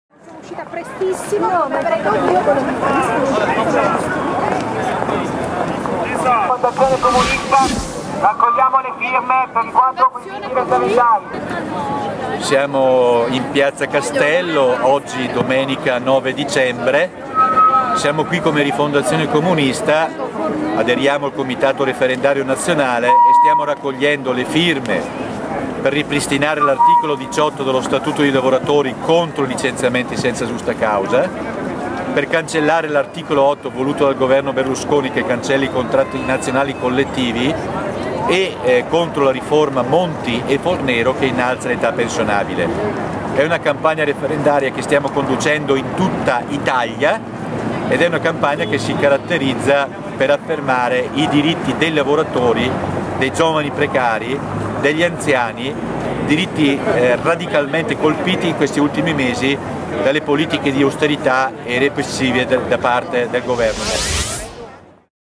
Comunisti torinesi in piazza Castello.
Comunisti torinesi in piazza Castello raccolgono le firme per referendum.
Costituzione italiana, articolo 18, 8